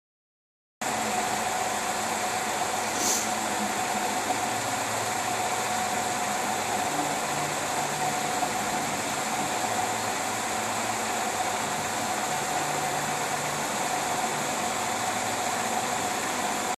GMC HHR Pilot/Escort Car
A vary short video showing the lights on the  my pilot car rotating its opposite directions.